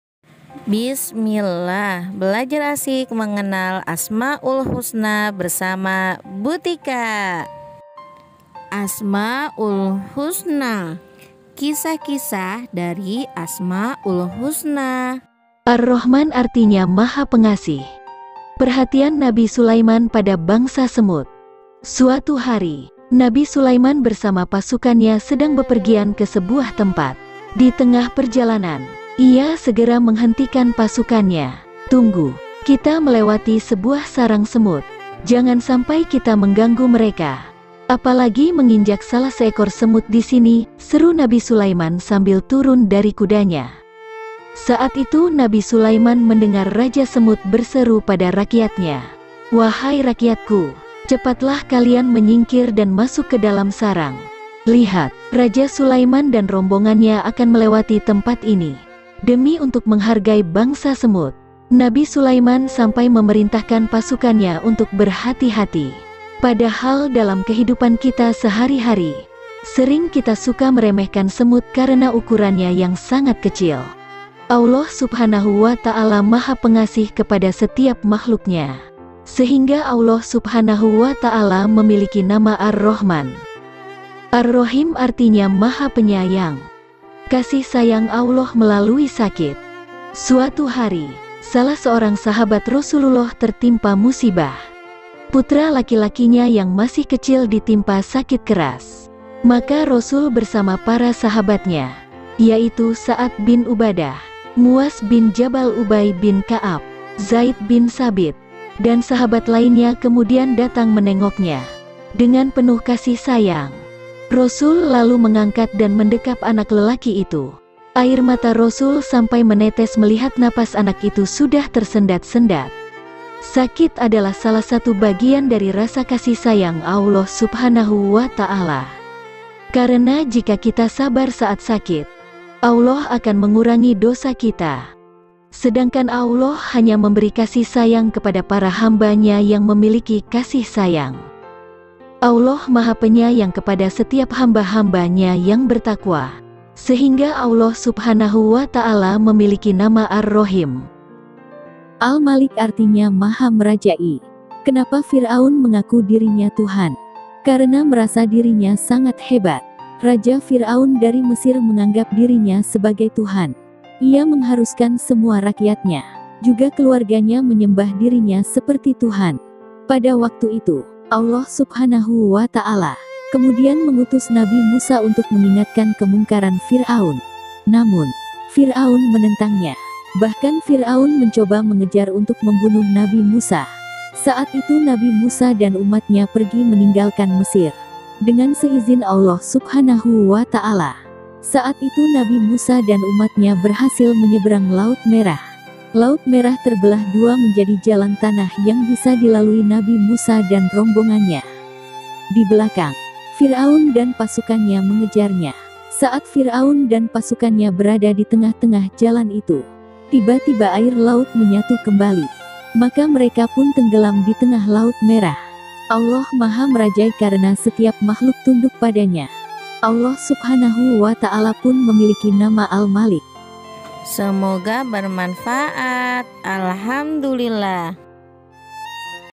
Belajar asik mengenal Asmaul Husna bersama Bu Tika | ArRohmaan | Ar Rohiim | Al Maalik
Asmaul Husna